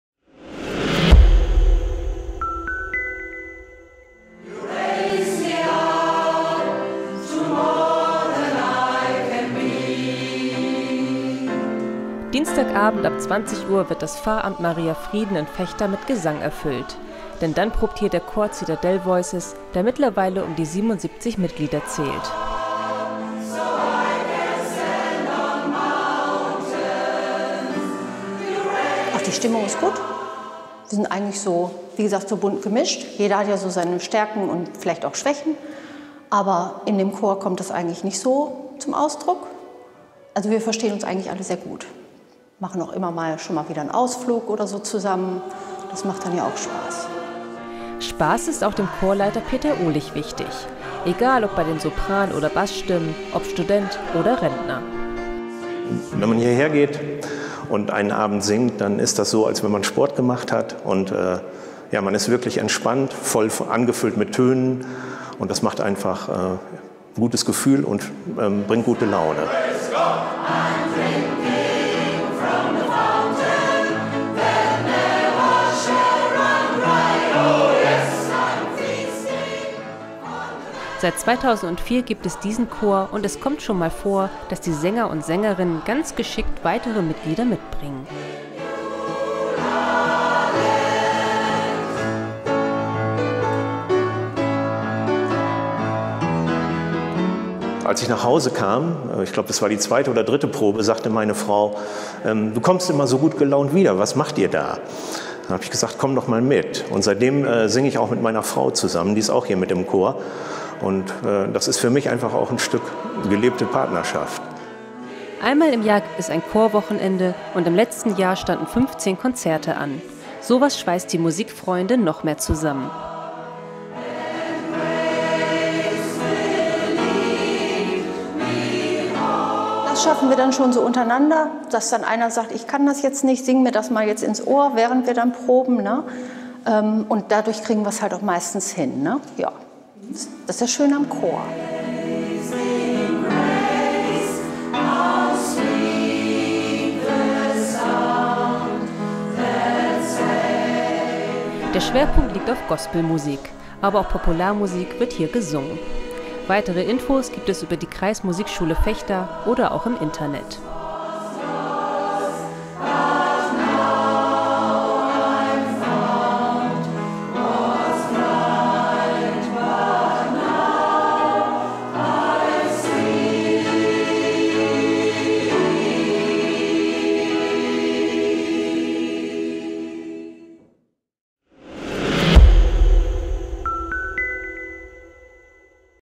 Musikserie Teil 15: Citadel Voices aus Vechta
Einen haben wir noch für euch: Teil 15 der Serie "Wir machen Musik". Den Abschluss macht der Chor Citadel Voices aus Vechta. Weitere Infos gibt es am 17. Oktober  in der gedruckten Ausgabe der Oldenburgischen Volkszeitung.